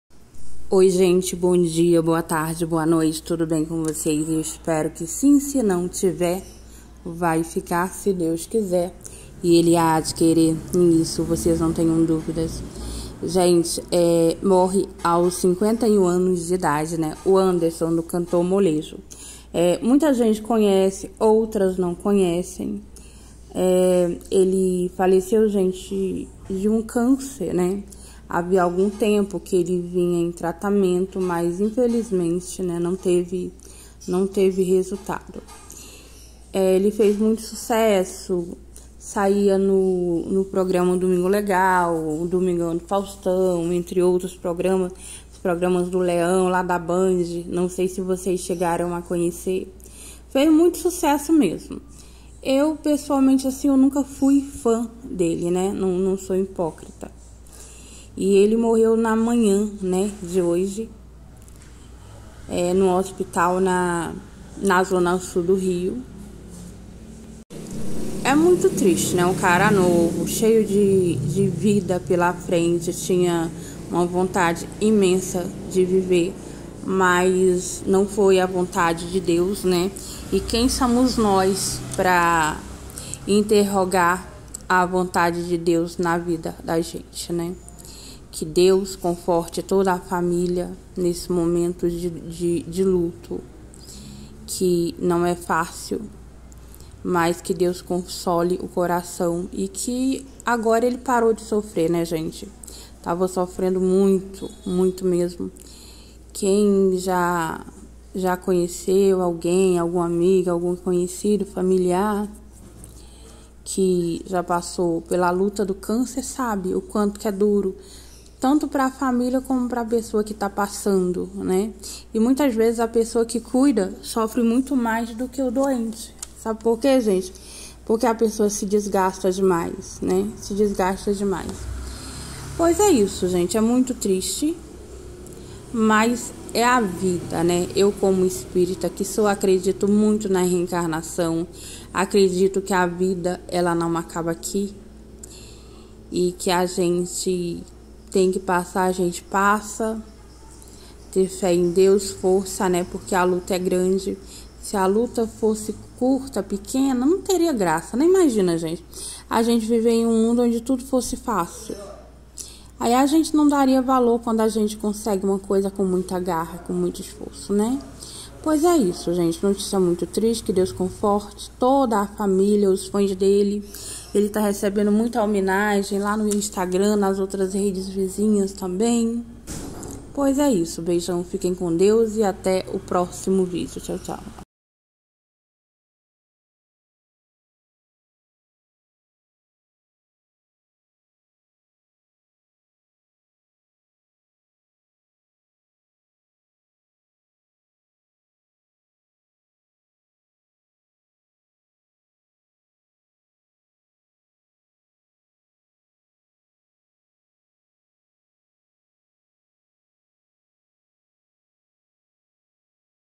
LUTO morre Anderson Leonardo cantor do Molejo 😢
Anderson Leonardo cantor do Molejo faleceu